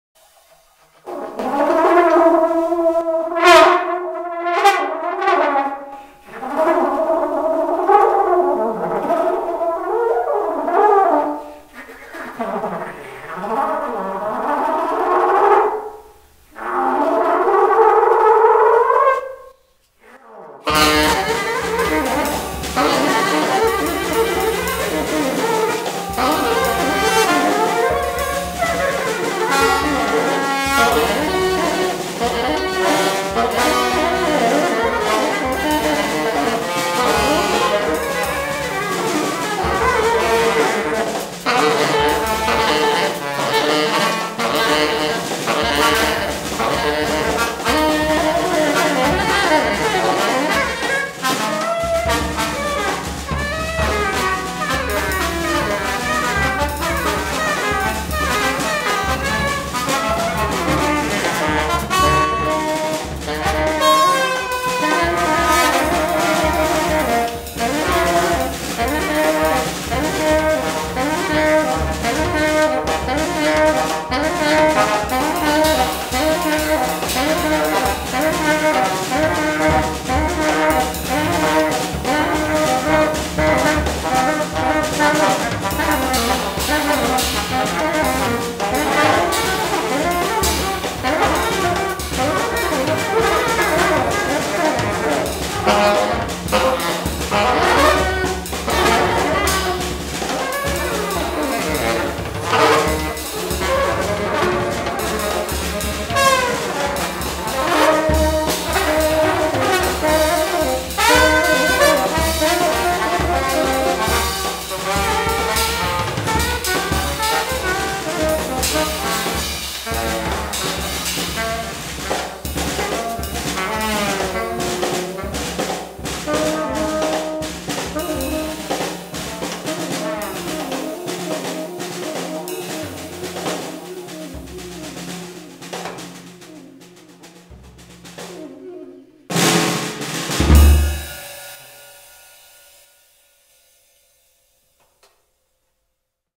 From the Basement#177 Slide Fast heArt Ensemble Guest: Scott Thomson
Slide Fast Guest; Scott Thomson Trombone Félix-Antoine Hamel tenor sax Yug Thouin Drums